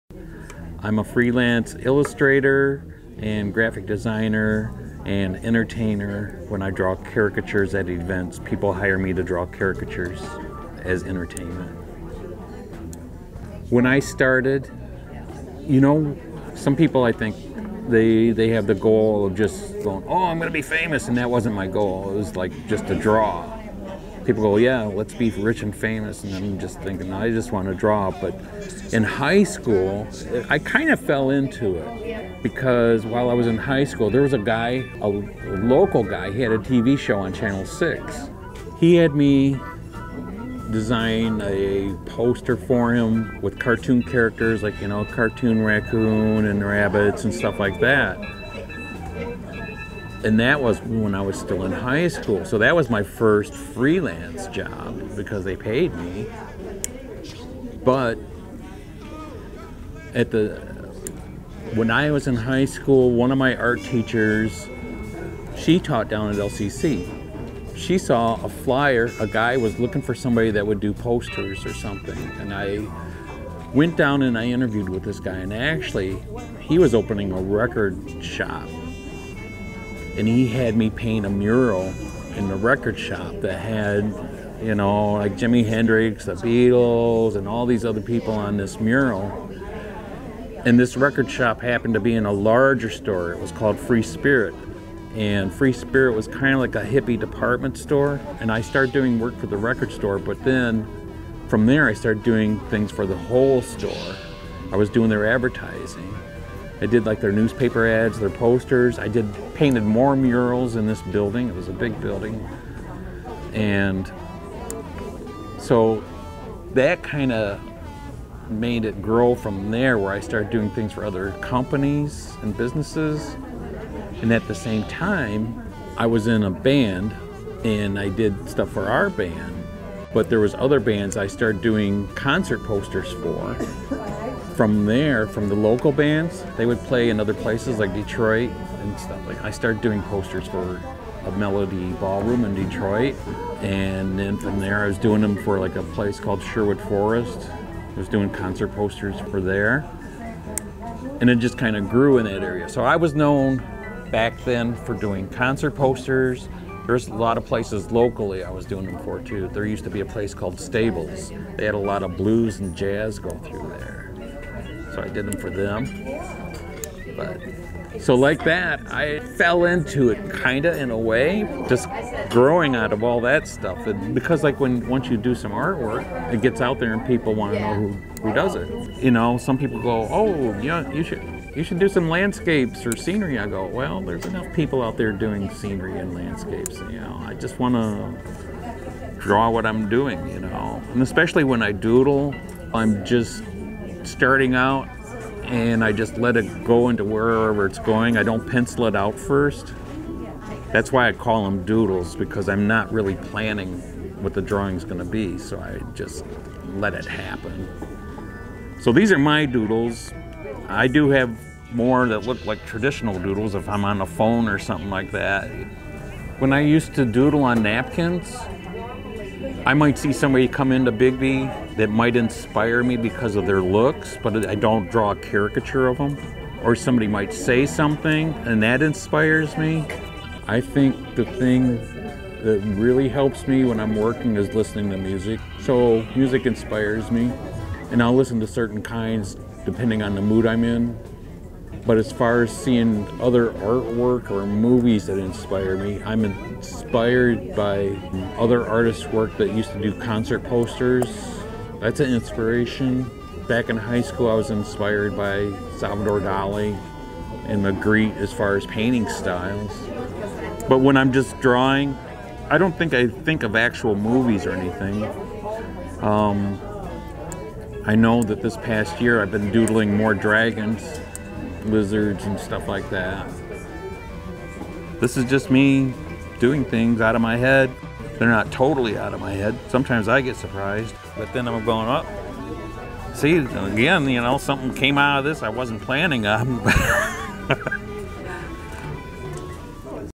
Dennis Preston Interview 2018
An interview I did as a junior intern in Fall of 2018 at HOMTV with Dennis Preston, An illustrator and entertainer well known in the art community in the Lansing area.